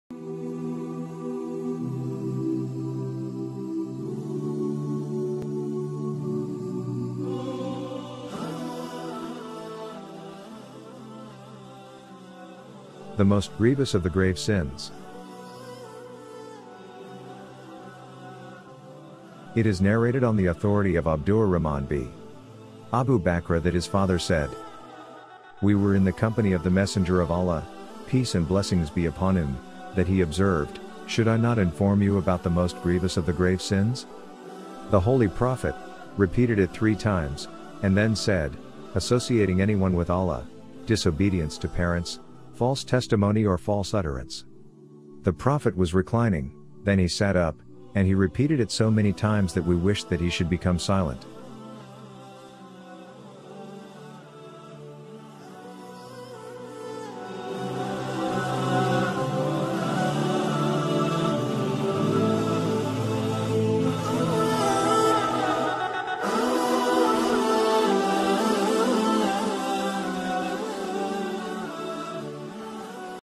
The most grievous and grave sins
Abu Bakrah (may Allah be pleased with him) reported: The Prophet (may Allah's peace and blessings be upon him) said: "Shall I inform you of the gravest of the major sins?" He repeated this three times, and then said: "Associating partners with Allah, mistreatment of parents." He was reclining, and then sat up and said: "And indeed the false statement and the false testimony." He kept repeating this so many times that we wished he should be quiet.